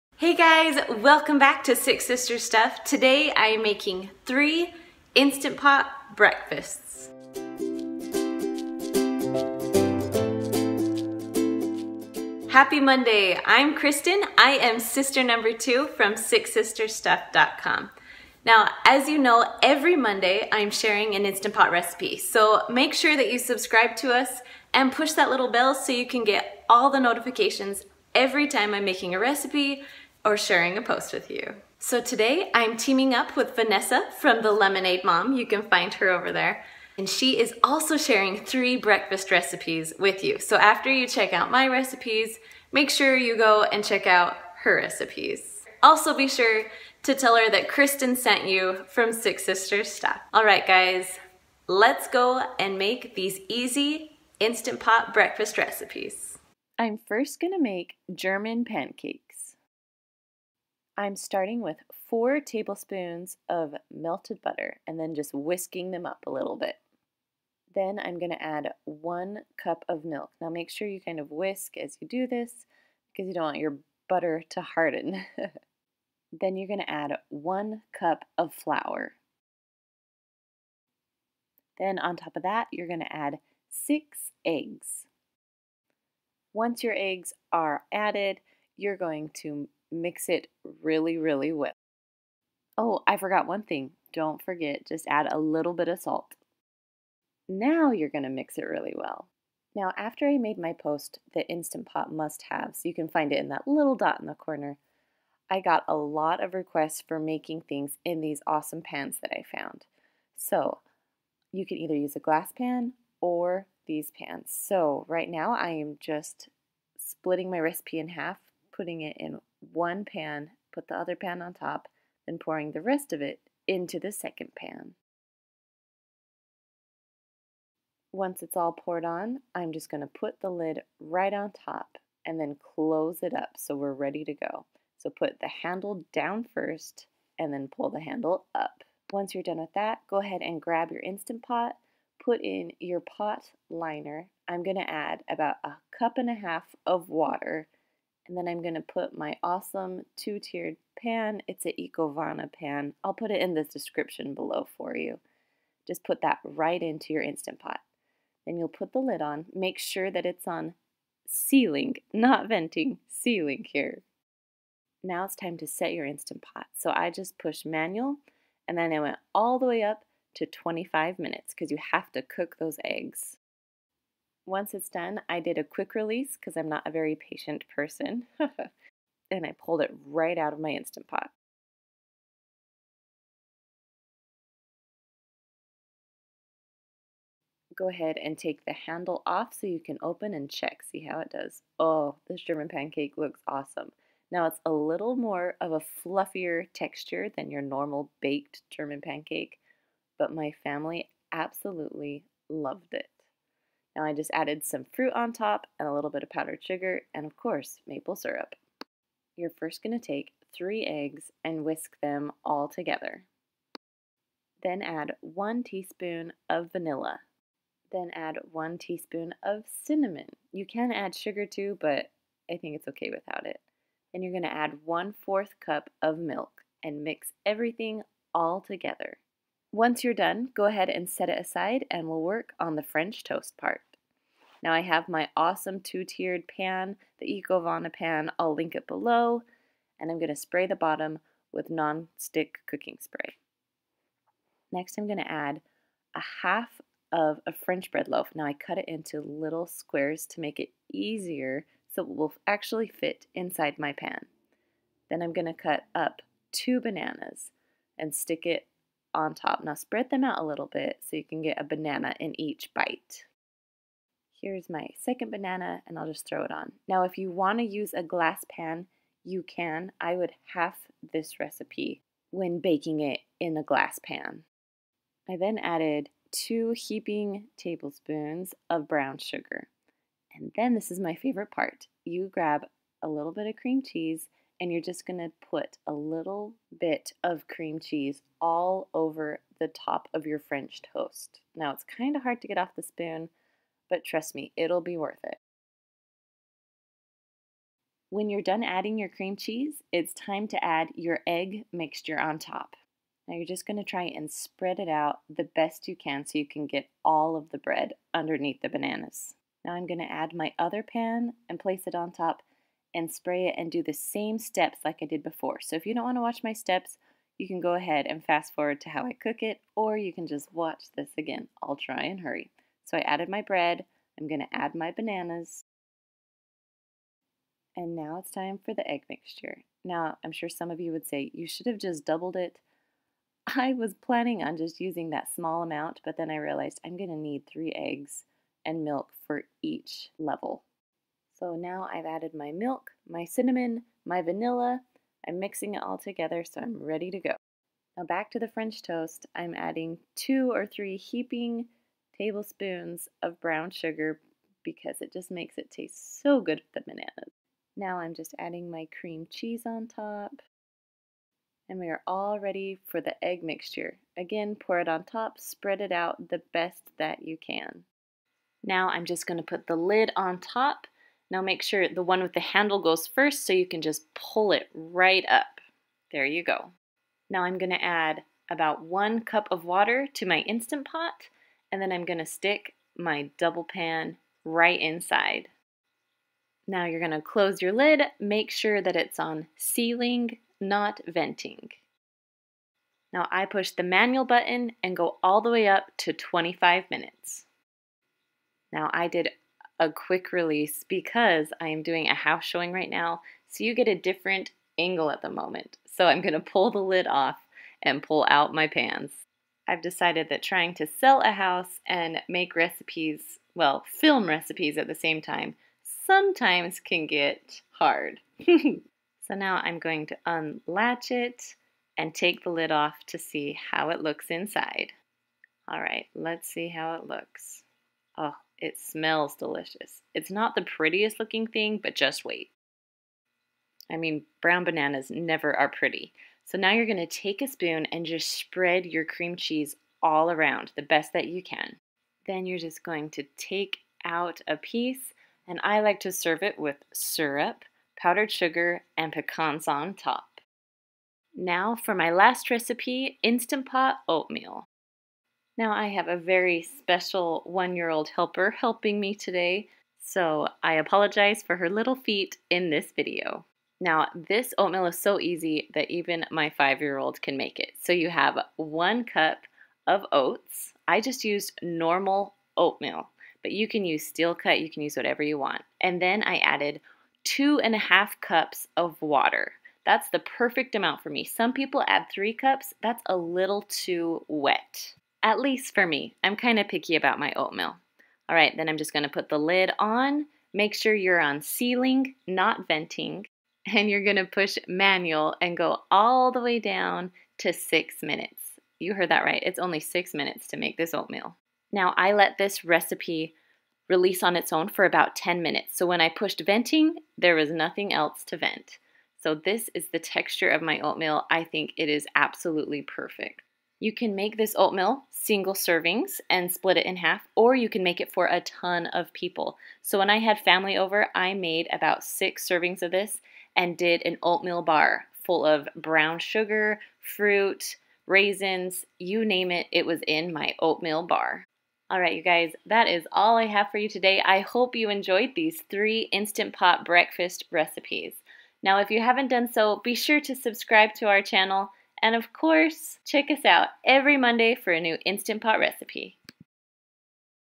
3 EASY Instant Pot Breakfast Recipes! for Beginners!
3 instant pot  breakfast recipes!
Meal 1german pancake recipe. Light and eggy with delicate, crispy edges.
Meal 2  French toast. Partly thanks to its simple base of eggs, milk, and bread, French toast is endlessly customizable.
Meal 3 is all about  oatmeal
Creamy, slightly nutty, and even a little sweet, oats are truly one of the most unique, humble superfoods we can eat.